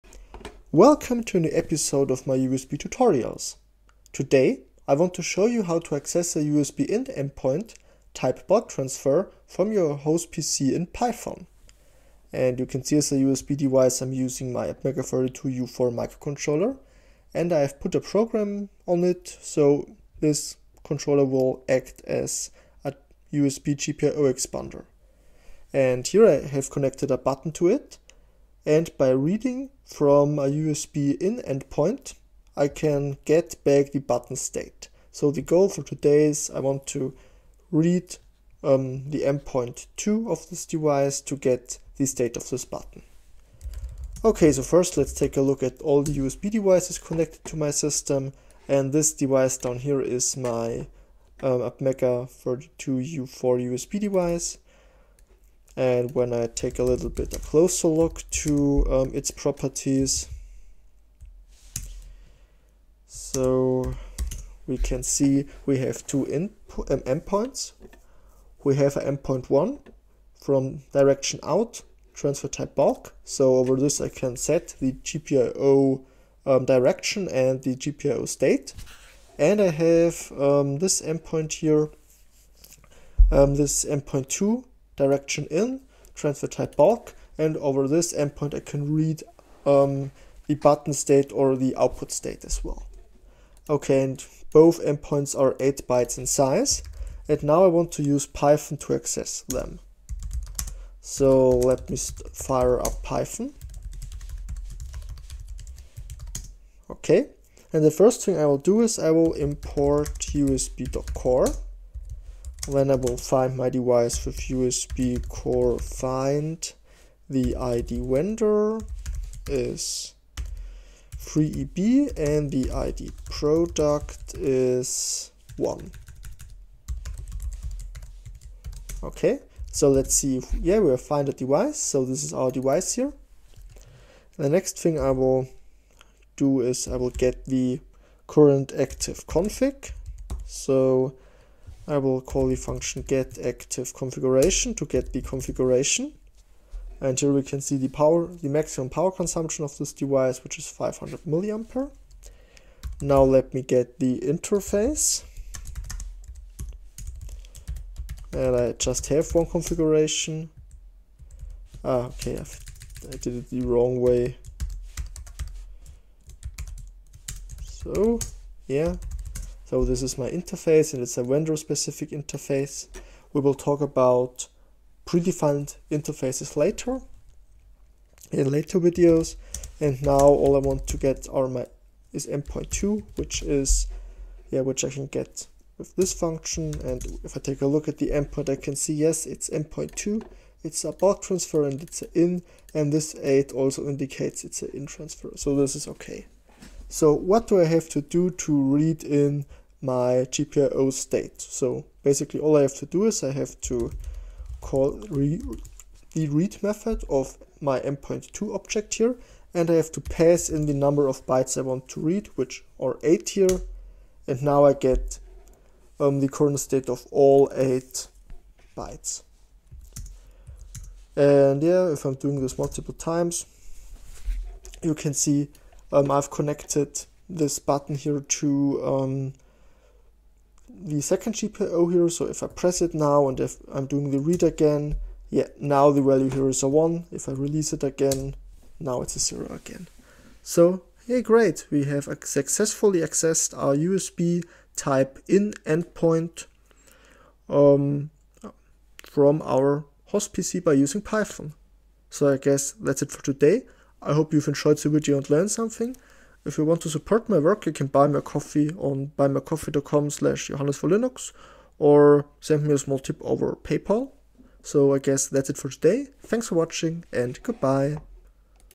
GNU/Linux & USB - Accessing USB In Endpoints in Python
Let's take a closer look at USB. In this series of tutorials we will learn how to implement an USB device on a microcontroller and interact with it over our GNU/Linux OS from user- and kernelspace.

I will show you how to access USB In Endpoints in Python by using pyusb.